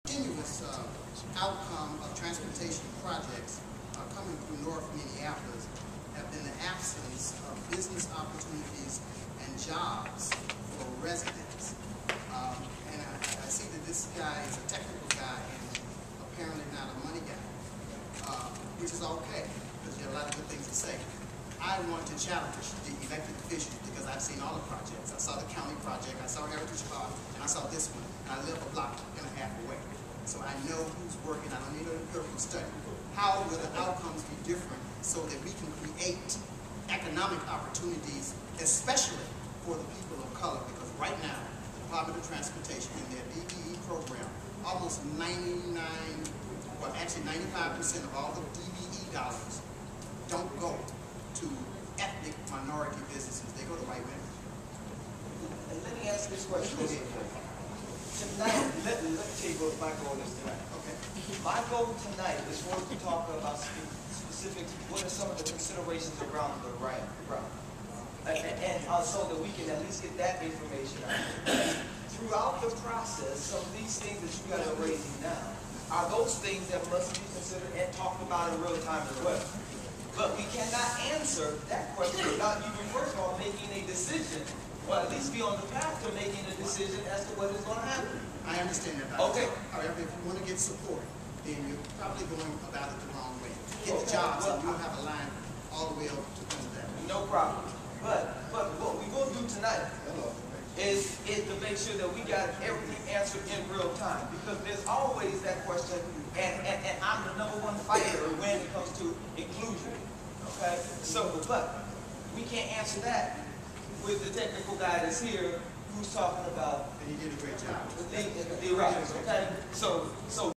Bottineau Transportation Town Hall - What about Jobs and Economic Opportunities?
A northside townhall meeting regarding the Bottineau LRT plans and development. Held on Jan 28th at UROC.